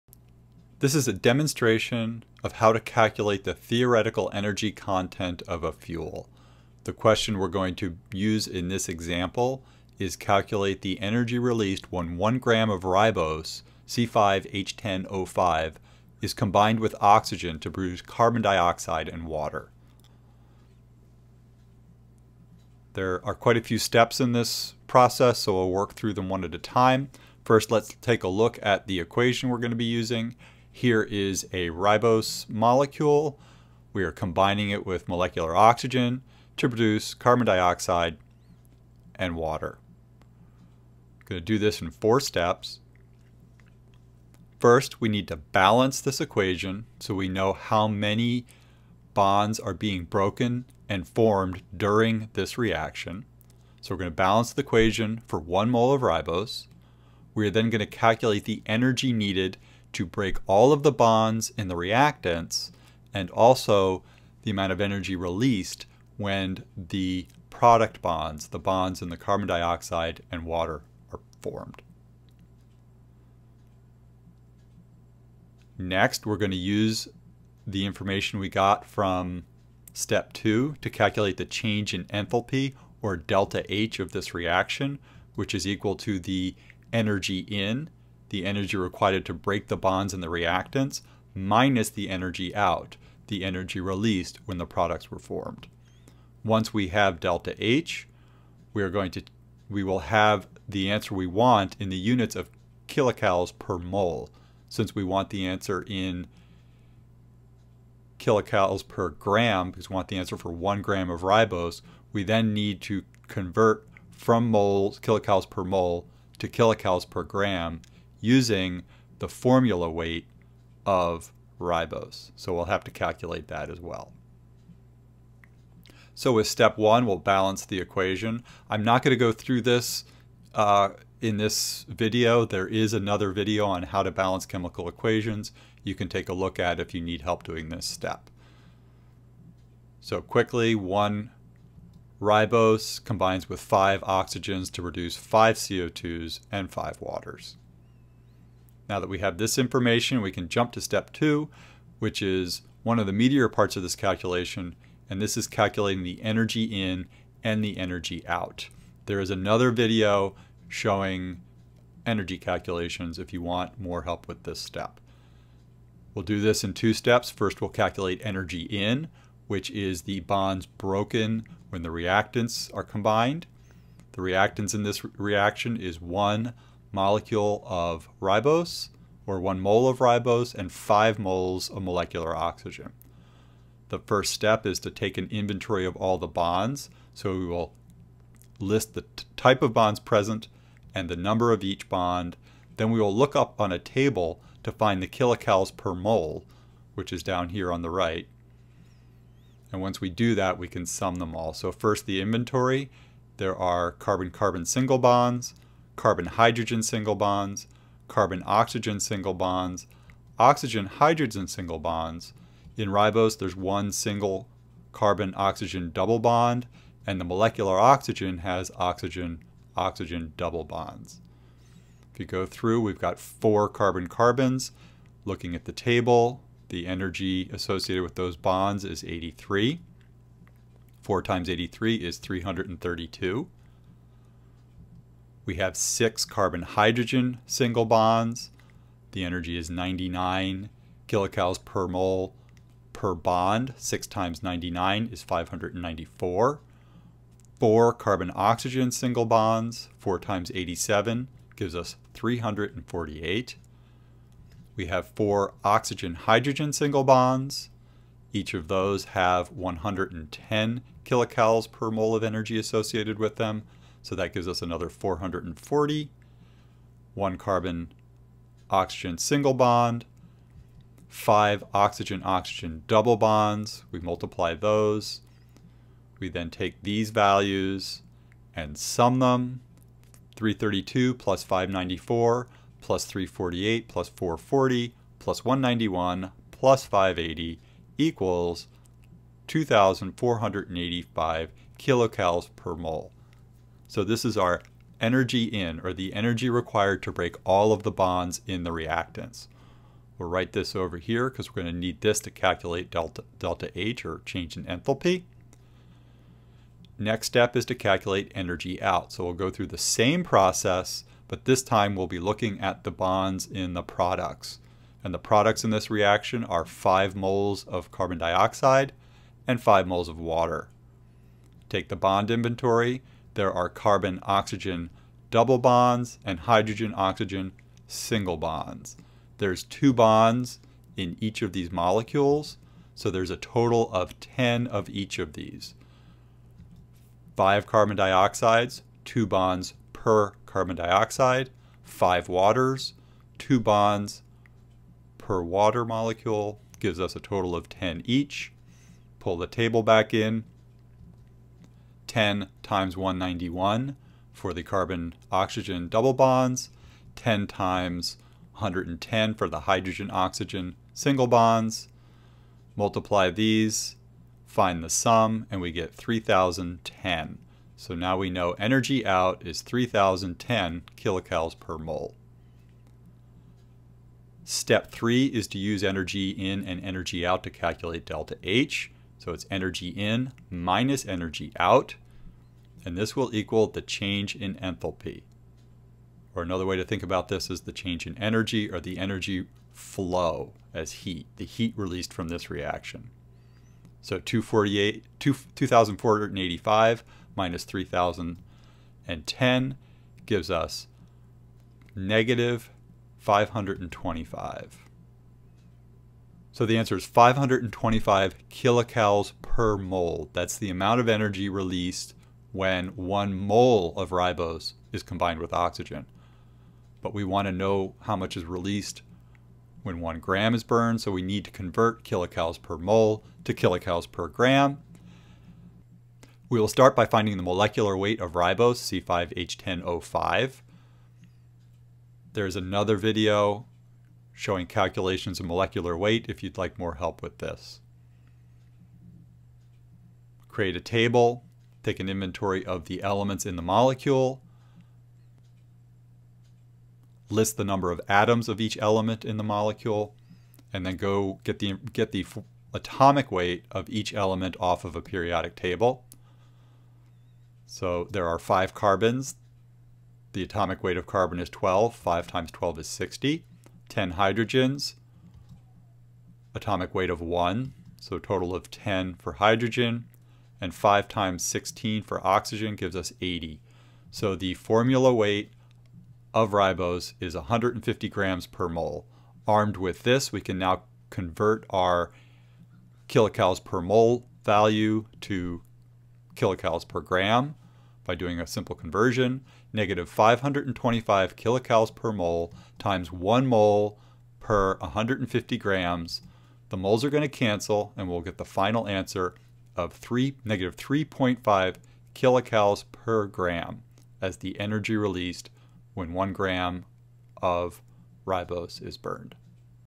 Example calculation of energy content of a fuel
Demonstration of how to calculate the energy content of a fuel using ribose as an example.